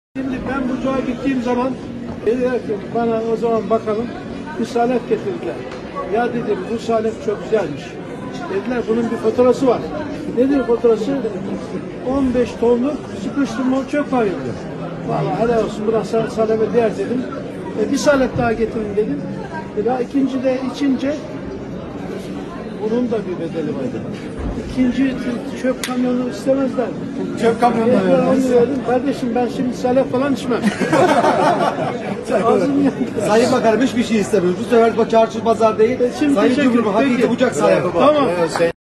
MEŞHUR BUCAK SALEBİ TBMM'DE
Bucak Salebi Ankara’da TBMM’de 2023 yılı bütçe görüşmelerinde ikram edildi.

Bucak Belediye Başkanı Emrullah Ünal, Burdur Milletvekili Yasin Uğur, Ak Parti Bucak İlçe Başkanı Mustafa Oğuz, Bucak Ticaret ve Sanayi Odası Başkanı Hasan Yalçın Meçikoğlu ve Ak Parti Bucak ilçe yönetimi TBMM'de yapılan ziyaretlerin ardından salep ikram ettiler.

Çarşı Pazar değil Hakiki Bucak salebi sloganı ile Başkan Hasan Yalçın Meçikoğlu, Orman ve Su İşleri Bakanı olarak görev yapan Afyonkarahisar Milletvekili Veysel Eroğlu'na Bucak salep ikramında bulundu.